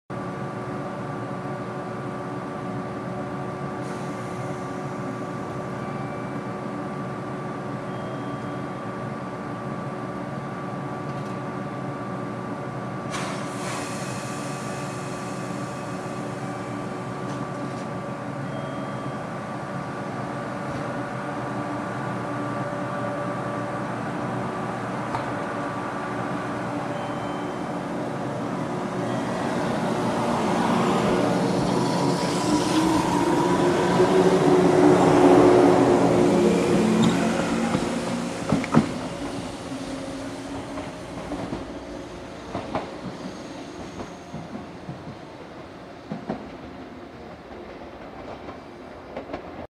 717系Hk-4編成 川内駅発車
撮影車両：717系Hk-4編成
感度自動設定のため後撮りしている際動きが鈍くなっています。